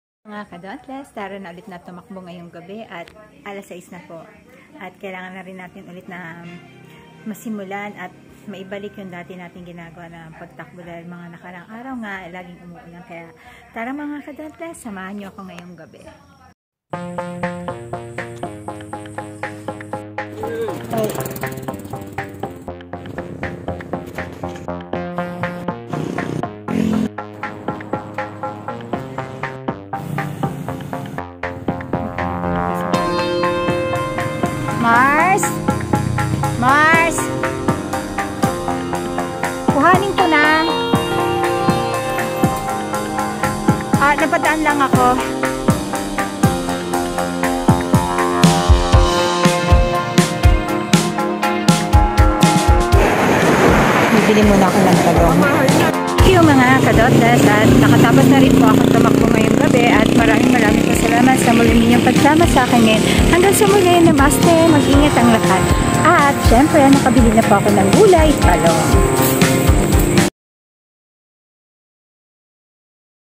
4.2Kms
10-09-23
6:15Kms
Magandang gabi mga kadauntless. Tara samahan niyo akong muli upang magjogging.